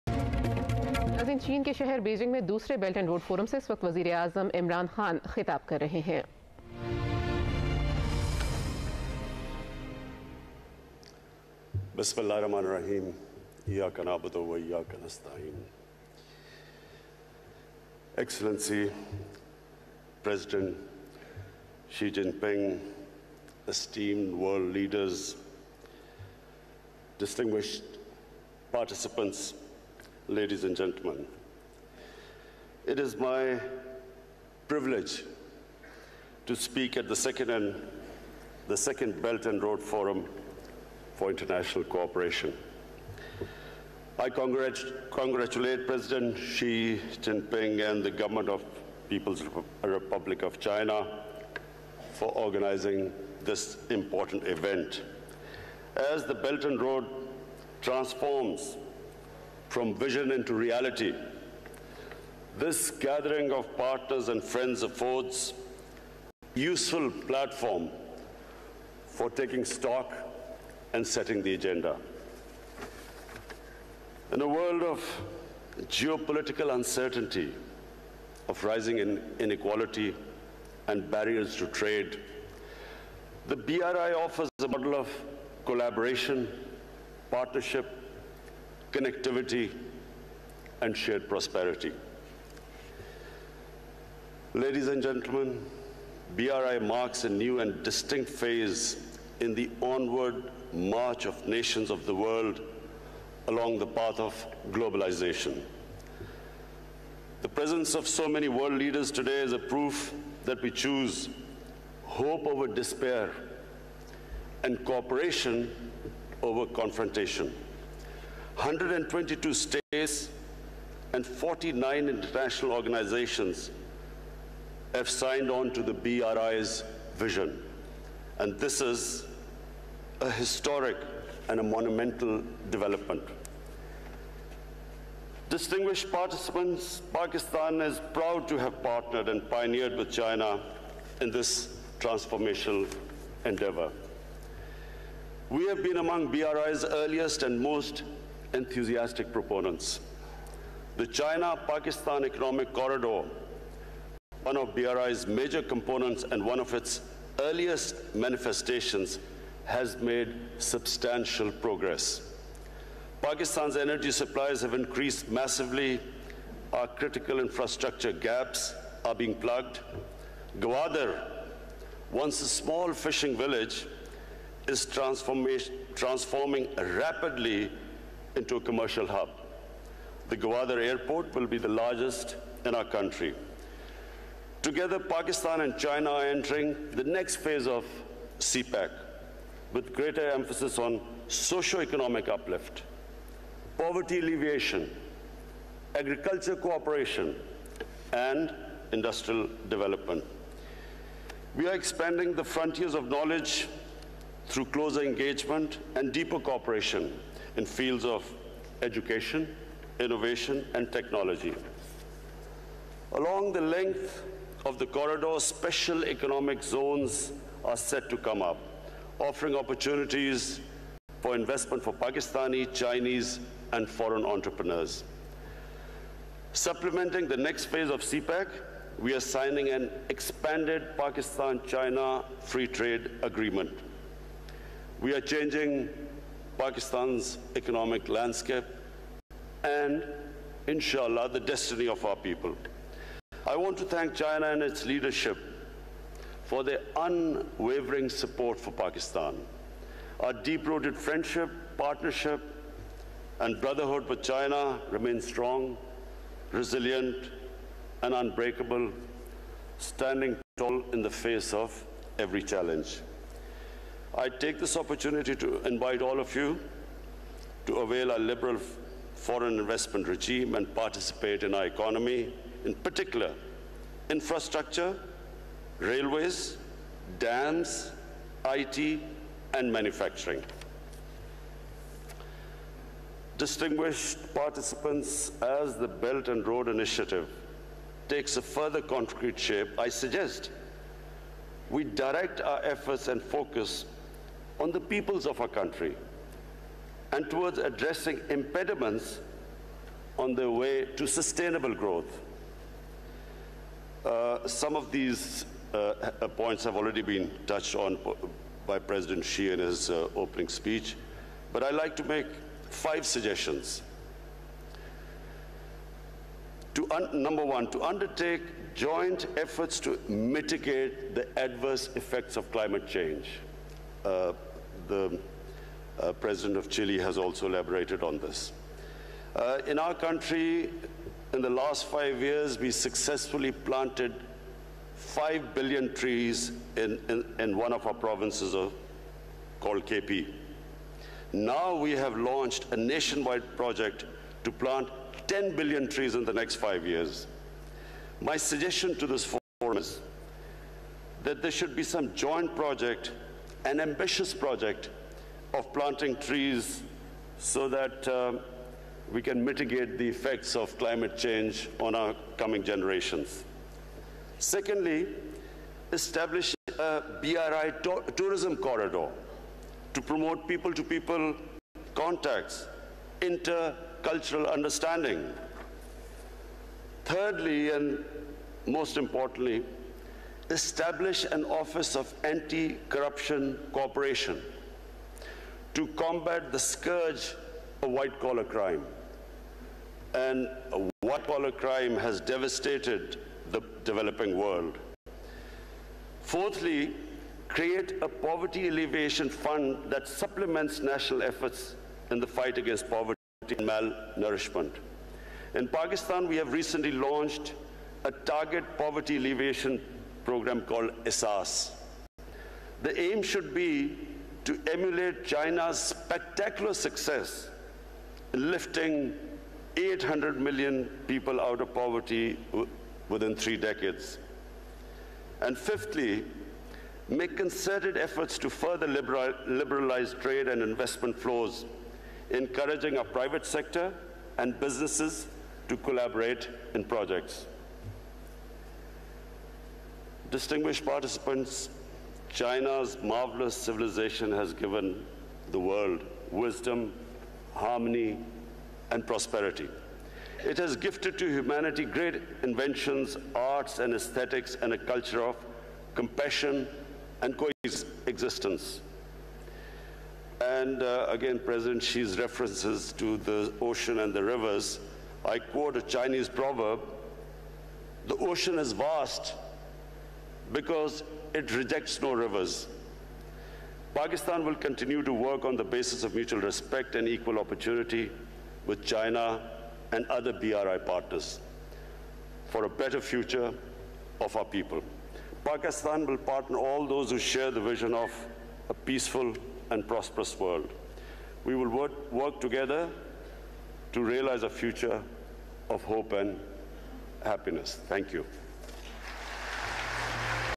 Imran Khan Speech today In China|Beijing
Prime Minister Imran Khan Addressing at the second Belt and Road Forum